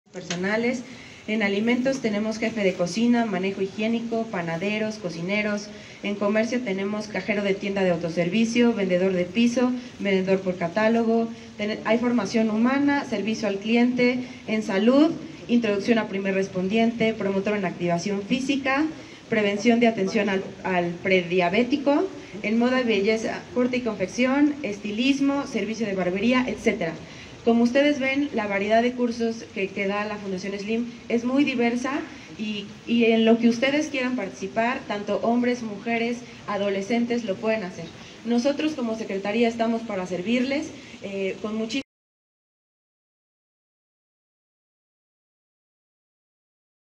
Cursos que "Fundación Slim" brinda a mujeres poblanas
La titular de la Secretaría para la Igualdad Sustantiva, Karina Romero Alcalá, comparte algunos de los cursos que "Fundación Slim" brinda a mujeres poblanas.